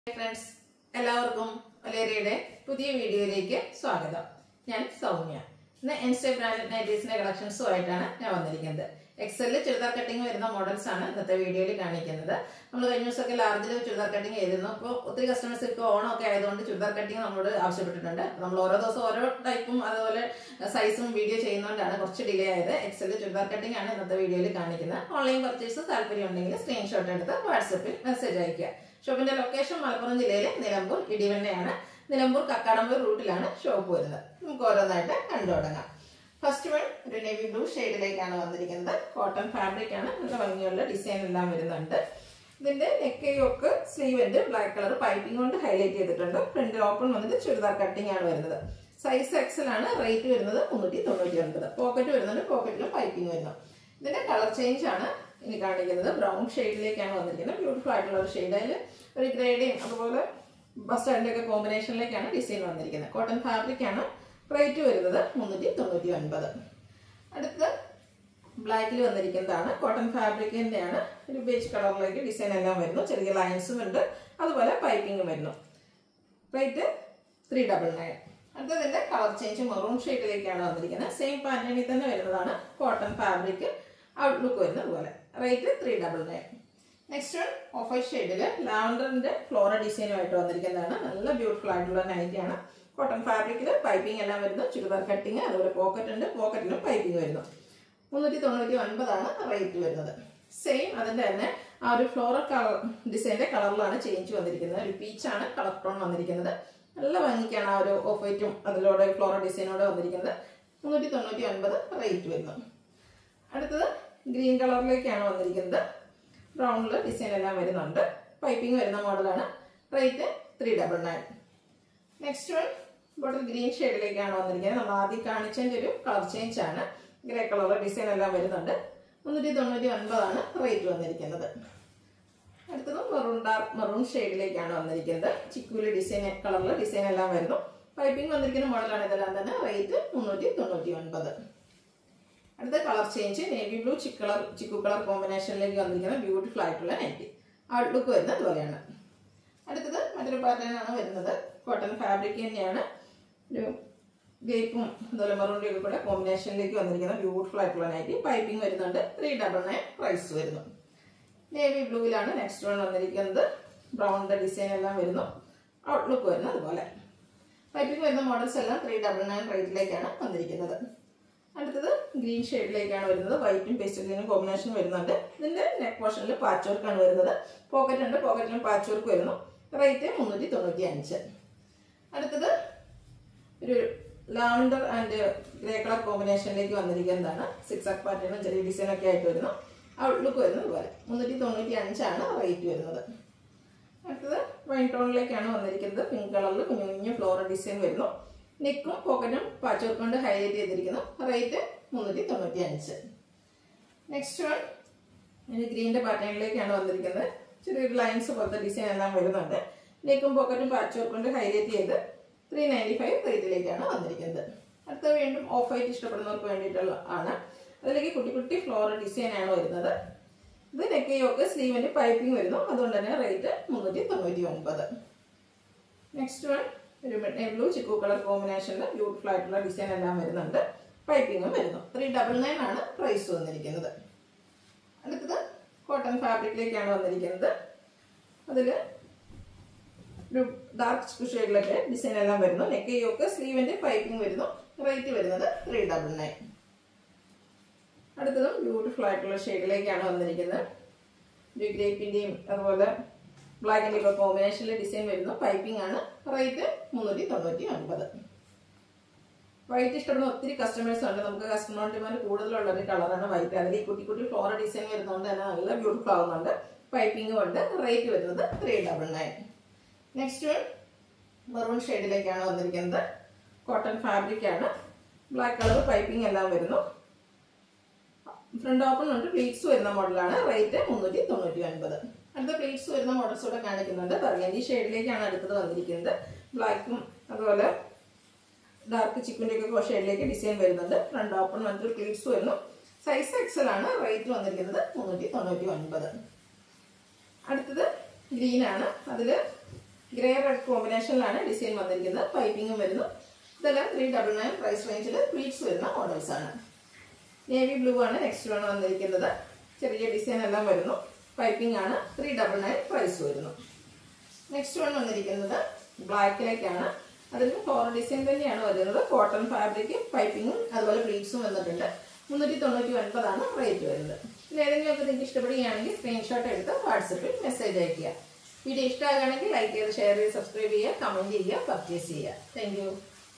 N Style XL Size (Pleated & A Line Cut)നൈറ്റികളുടെ അടിപൊളി കളക്ഷൻ 🌹Booking : 9074248160, 8848089032
N Style  Nighties
Budget Nighties
Size  : XL
Rate : Starting @ 395/-
Material : Cotton & Rayon

Payment : Google Pay, Bank Transfer, Phone Pay

Location : Edivanna
                   Nilambur
                   Malappuram
                   Kerala
Pincode  : 679329